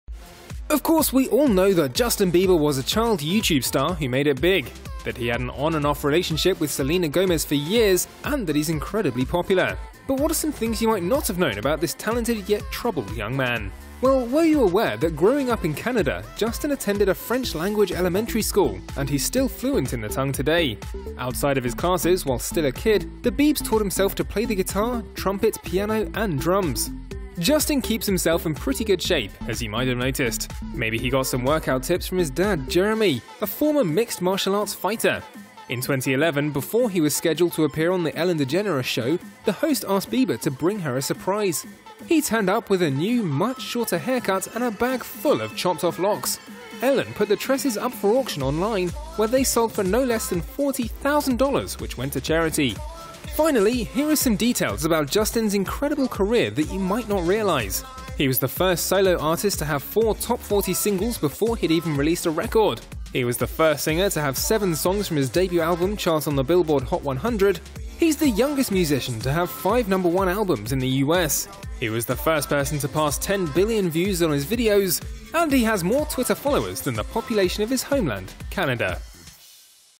The Stars' Best Kept Secrets: Justin Bieber
Of course we all know that Justin Bieber was a child YouTube star who made it big, that he had an on and off relationship with Selena Gomez for years, and that he's incredibly popular. But what are some things you might not have known about this talented yet troubled young man?